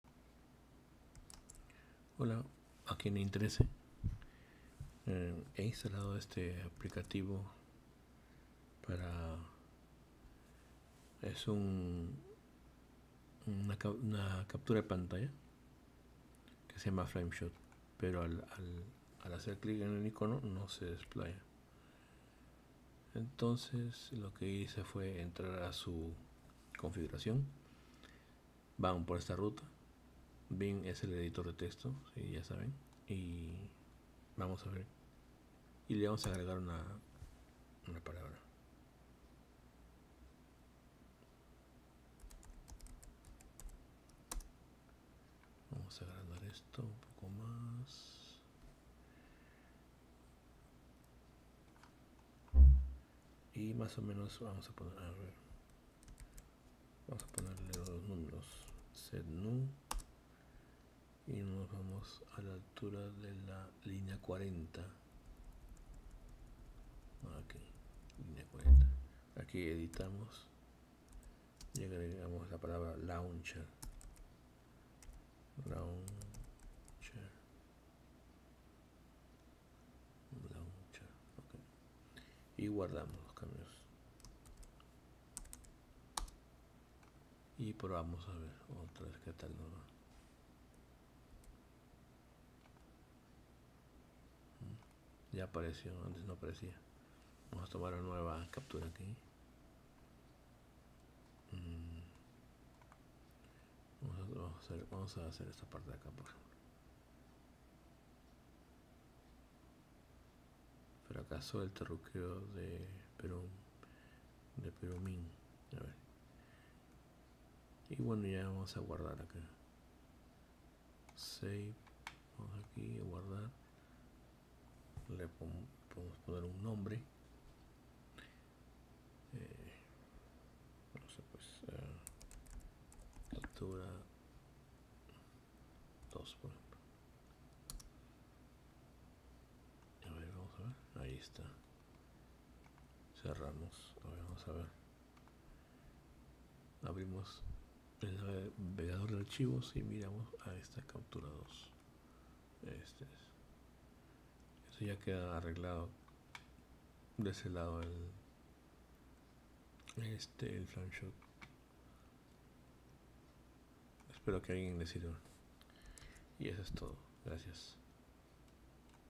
Solución de flameshot, no iniciaba
Hola a quien le interese, al hacer click en el icono de flameshot la aplicación no se desplyaba o no iniciaba, agregé en la linea 40 la palabra launcher y luego guarde cambios.